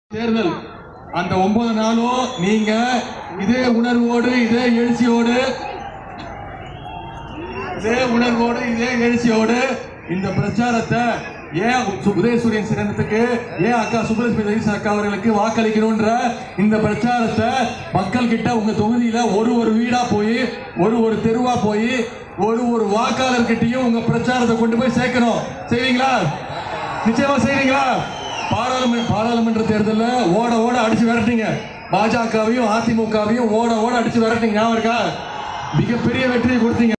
UDHAIYANIDHI STALIN IN MODAKKURICHI TO SUPPORT THE DMK CANDITATE SUBBULAKSHIMI JAGADEESAN (2021)
udhaiyanidhi stalin in modakkurichi consituency to support the DMK CANDIDATE subbulakshmi jagadeesan திமுக துணை பொதுசெயலாளர்